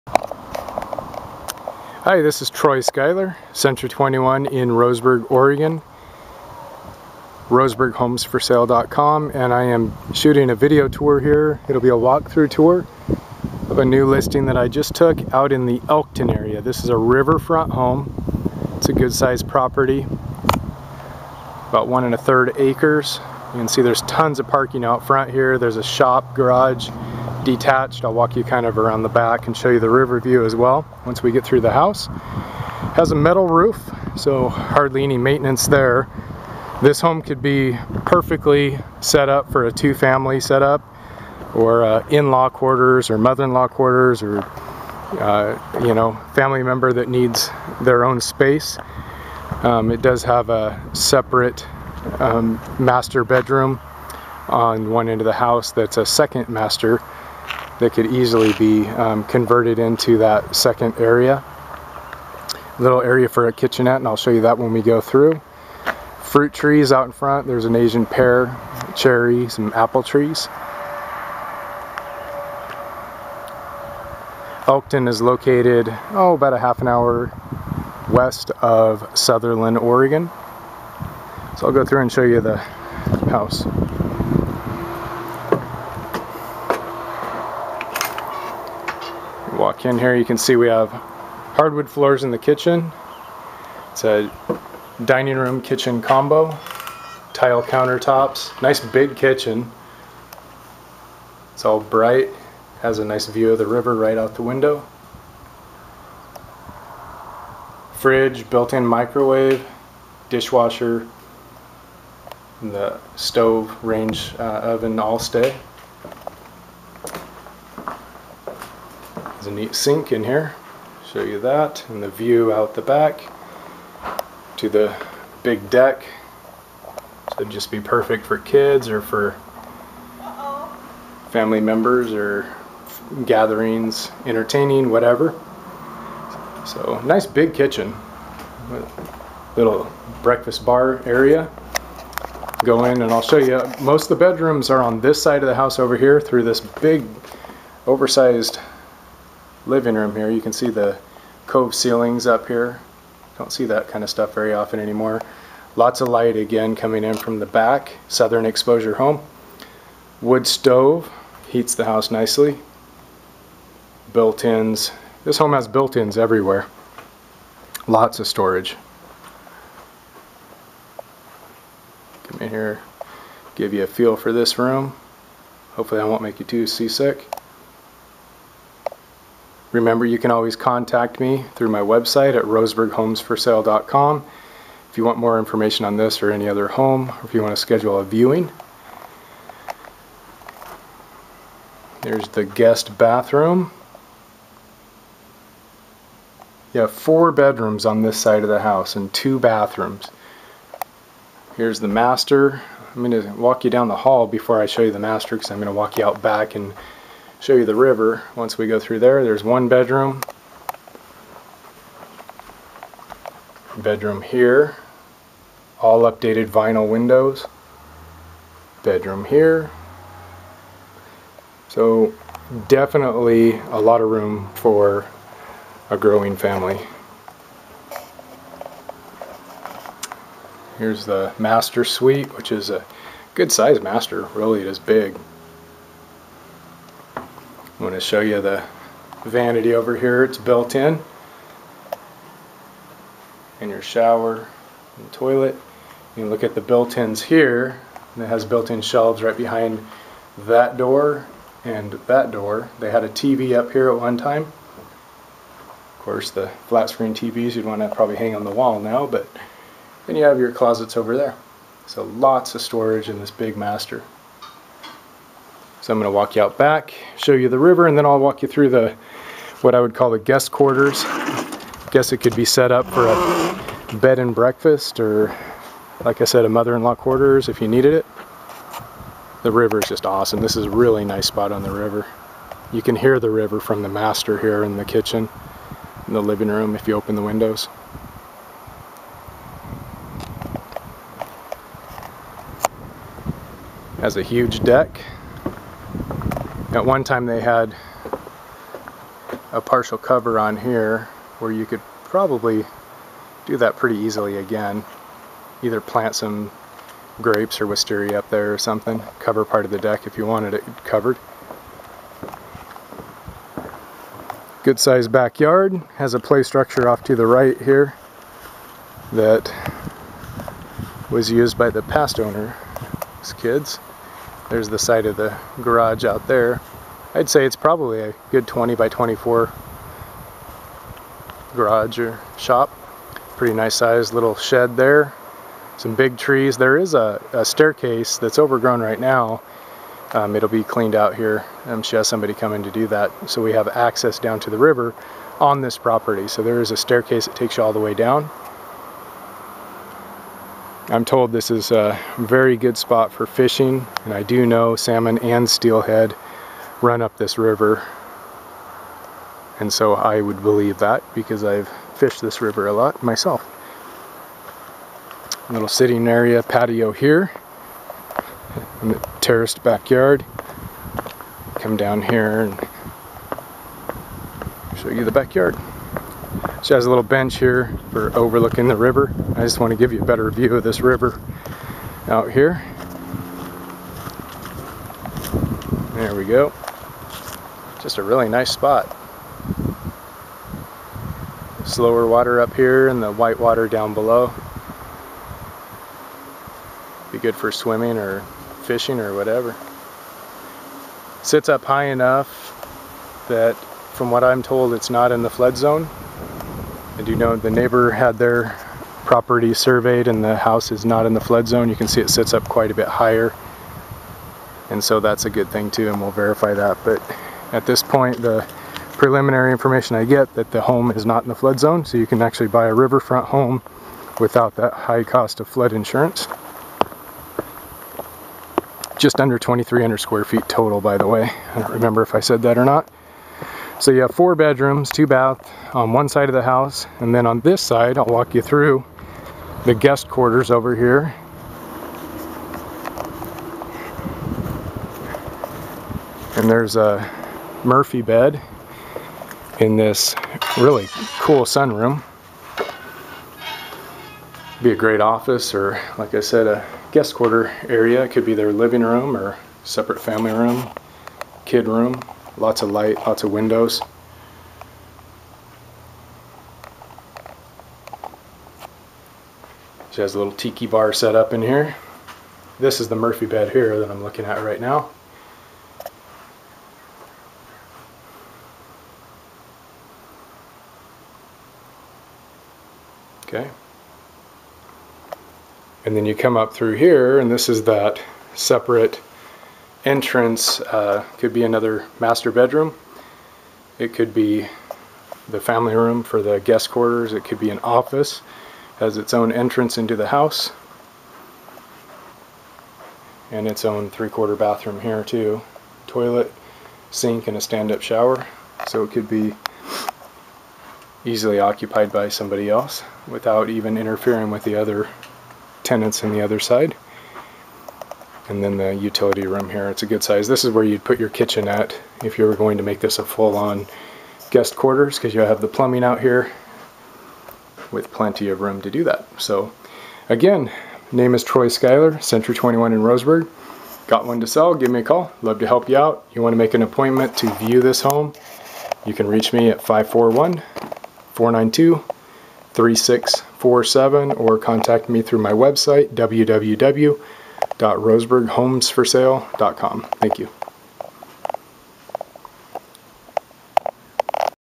Douglas County OR Riverfront Home For Sale @ Troy Schuyler
4 Br. Riverfront Home For Sale | Elkton Oregon Riverfront.

Amazing spot on the Main Umpqua.  This home is located in the small but cute town of Elkton Or. It is right about half way between Sutherlin OR and the coast.  It is also close to Loon Lake.

This home would make an easy 2 family set-up.  It has sep. 1 Br. living quarters... just needs a kitchenette installed. (plumbing is there)

The home itself has a large living room large master, great sunroom, and classic 50's kitchen.  All of which have a spectacular view of the river.  You can actually open the windows and hear the water.

4 Br. 3 Ba.
2266 SFT.
1.15 Riverfront Acres

Troy Schuyler
CENTURY 21 The Neil Company Real Estate
Roseburg OR 97471